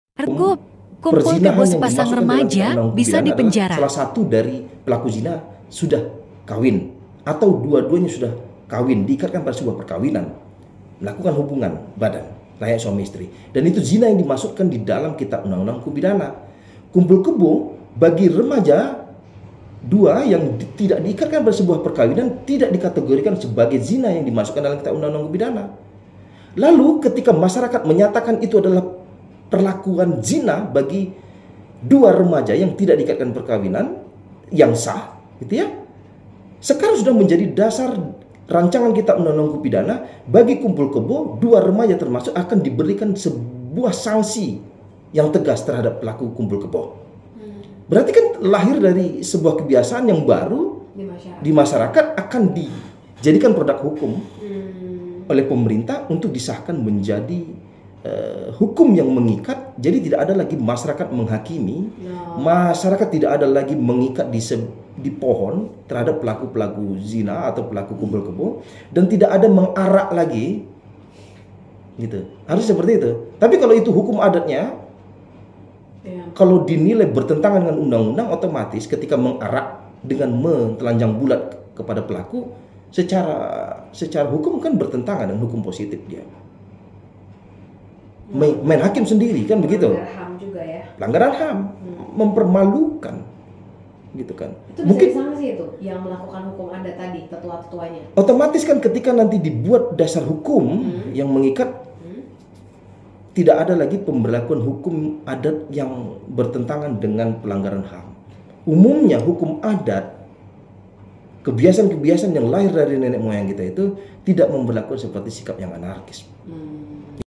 Dalam RKUHP, "kumpul kebo" sepasang remaja bisa dipenjara | KANTOR HUKUM PARLINDUNGAN, SH. MH. CLA
Dalam Rancangan Kitab Undang-Undang Hukum Pidana (RKUHP), "kumpul kebo" sepasang remaja bisa dipenjara.

KANTOR HUKUM PARLINDUNGAN, SH. MH. CLA